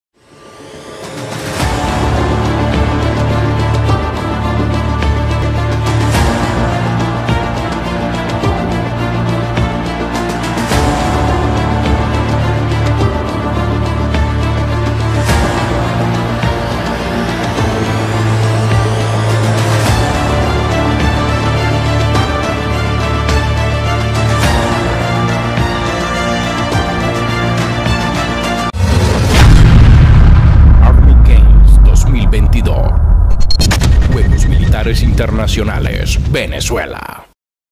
Equipo de Biatlón de Tanques ¡Triunfar por Venezuela! Army Games 2022
🇻🇪 Desde el Fuerte Alabino, en la Federación de Rusia, exaltamos el legado de nuestro Comandante Chávez por continuar llevando la marcha de nuestra juventud militar que con voluntad de vencer sigue el equipo de Biatlón de Tanques. ¡Triunfar por Venezuela!